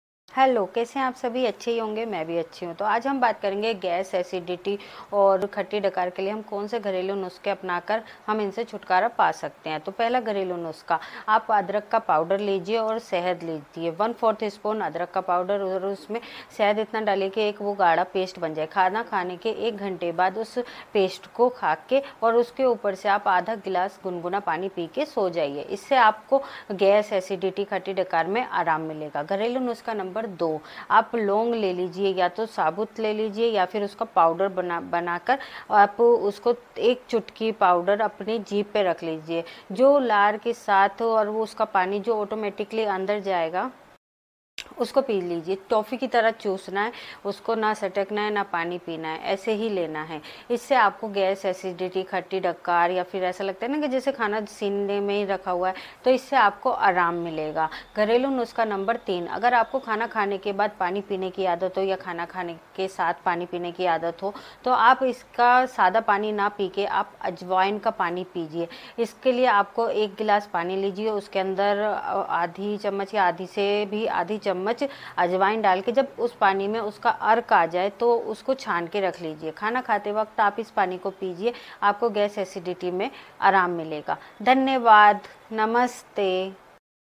पेट में बहुत ज्यादा गैस बनने पर क्या करें?
एसिडिटी से तुरंत राहत पाने के लिए क्या करें?
गैस कब्ज एसिडिटी के लिए घरेलू उपाय?
एसिडिटी की सबसे अच्छी दवा कौन सी है?